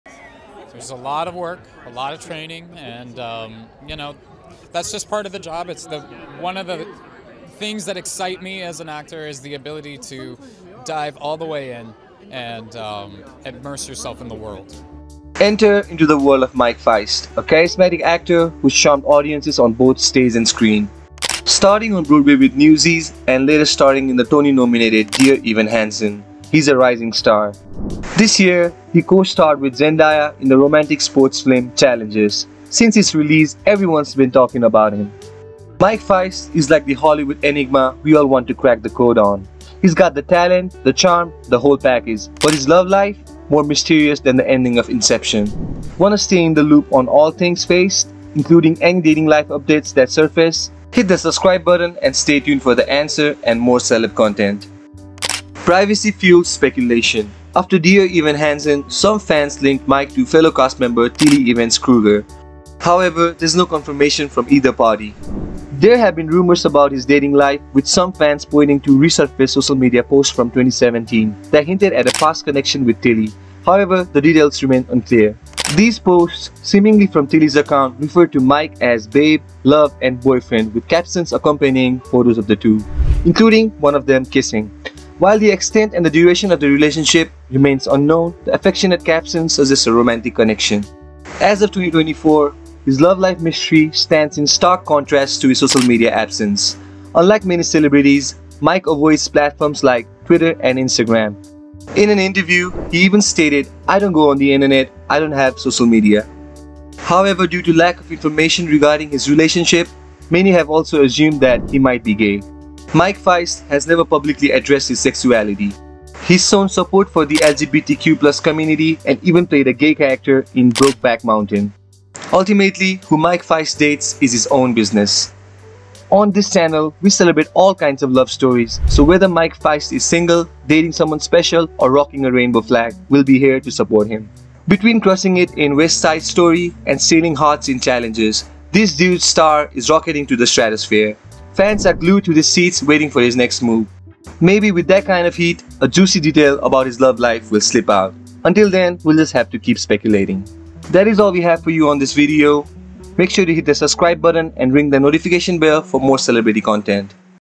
Is Mike Faist Keeping a Big Dating Secret?
Mike Faist, known for his roles on Broadway and in films like "Dear Evan Hansen" and "Challengers," has a mysterious dating life that intrigues fans.

Speculation about his relationships with co-stars like Tilly Evans-Krueger has circulated, fueled by past social media posts. Despite this, Faist maintains a private life, avoiding social media.

While some speculate about his sexuality, Faist has never publicly addressed it. Fans eagerly await updates on his personal life while celebrating his career achievements.